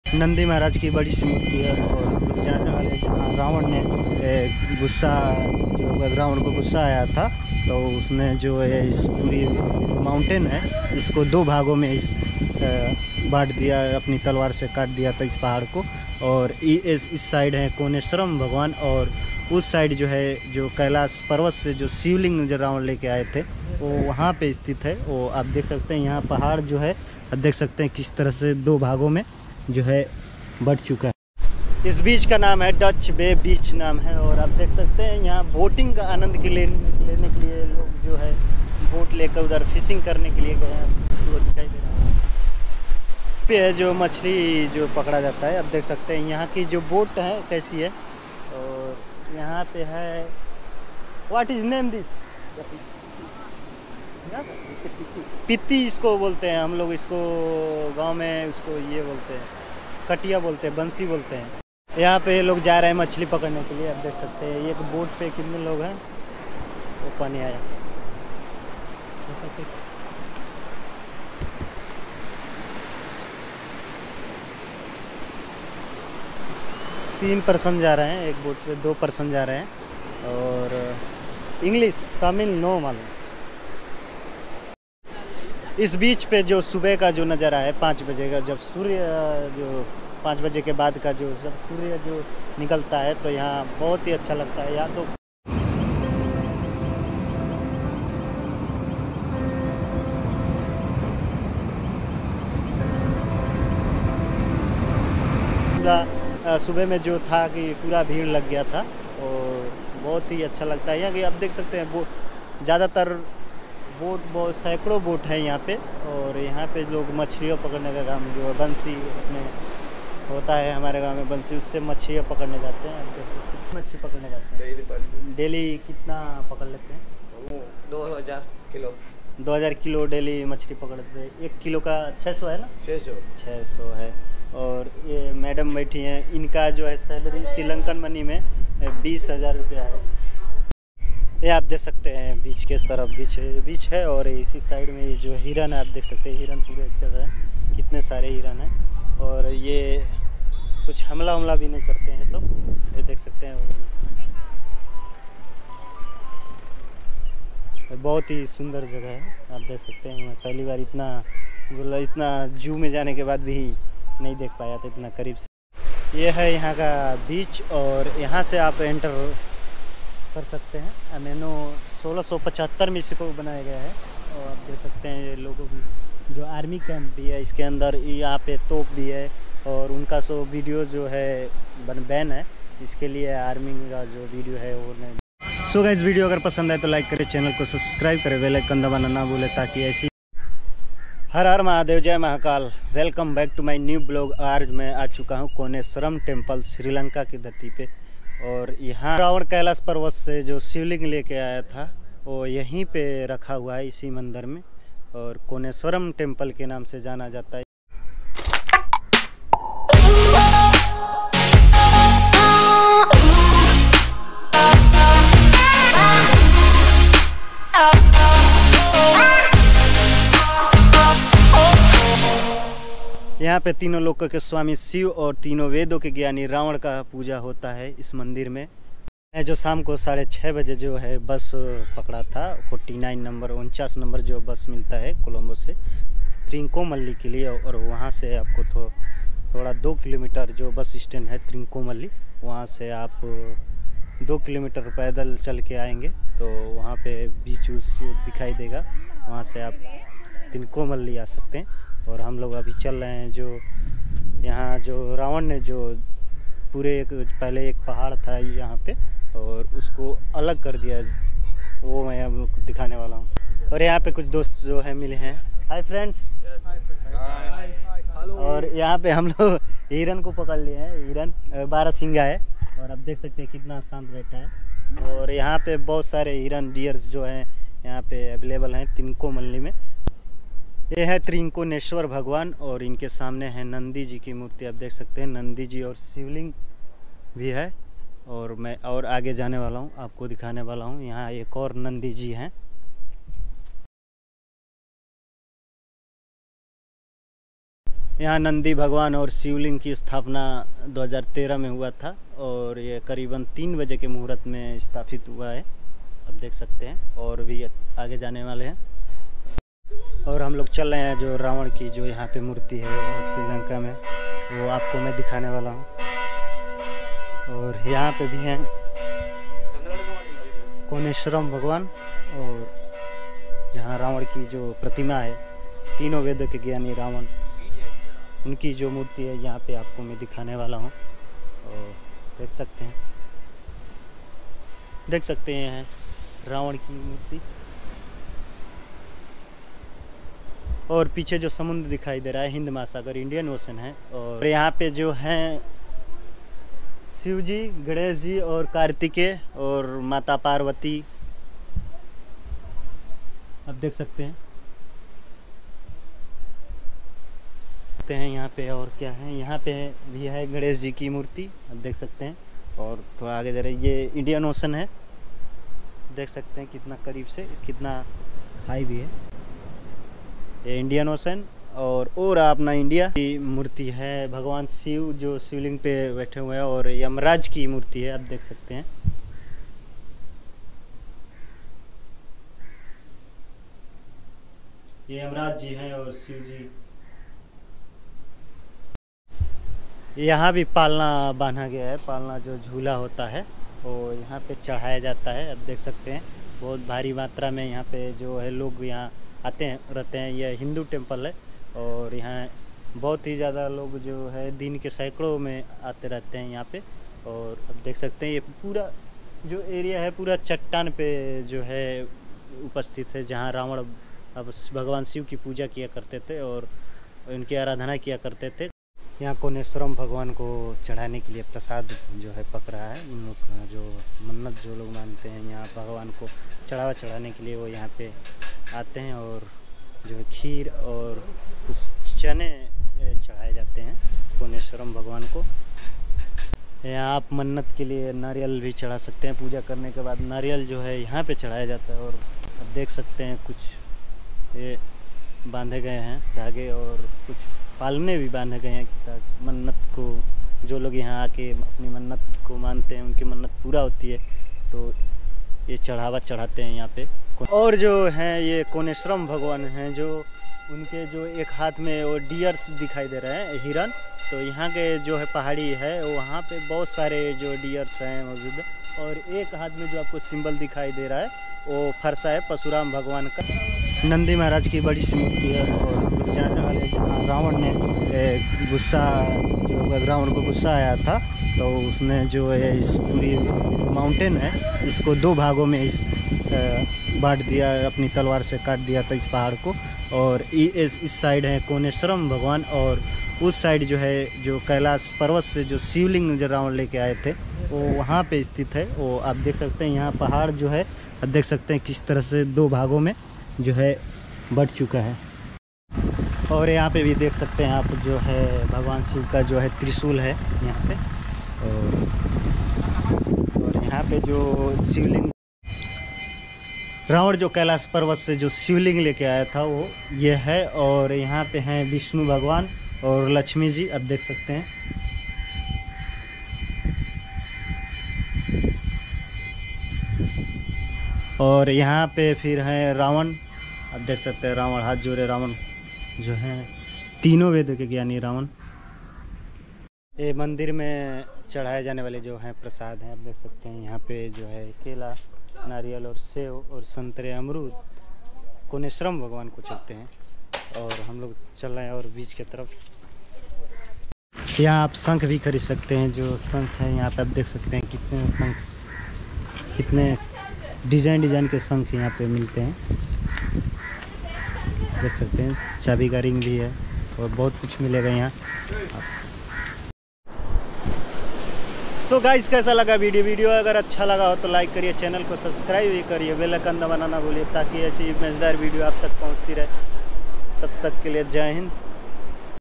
रावण का मंदिर श्रीलंका। Ravan Ka Mandir Sri Lanka. kelash parwat se sivling laya tha
Ravan Ka Mandir
sivling
Trincomalee Srilanka